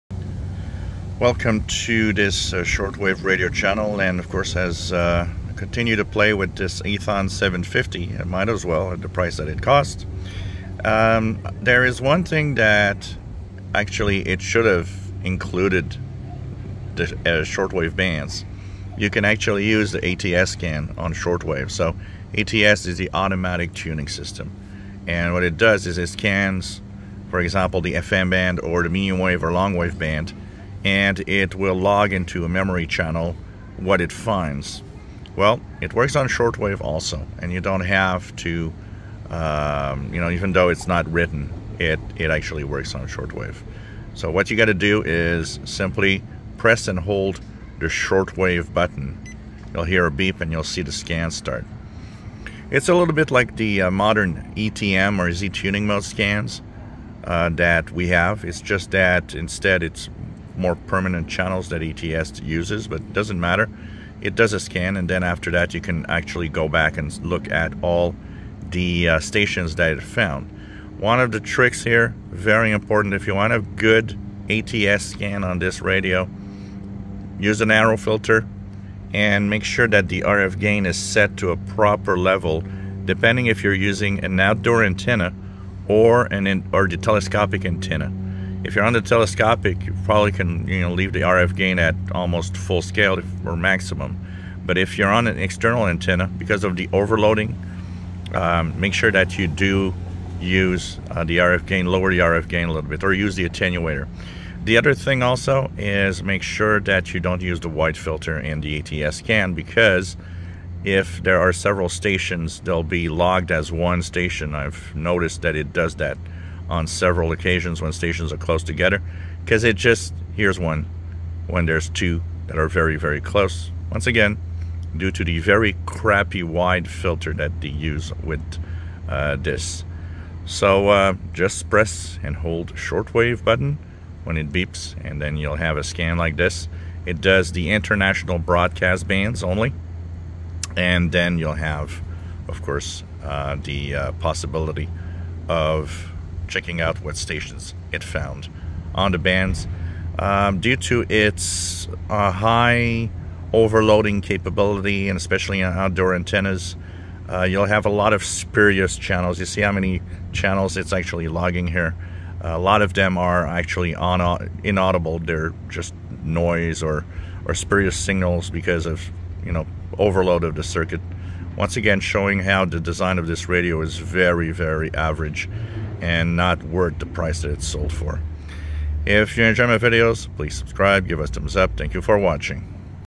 Eton Elite 750 ATS scan also works on Shortwave bands to find International broadcast stations
The way they show it, it would seem to work only on MW LW and FM but it also works on SW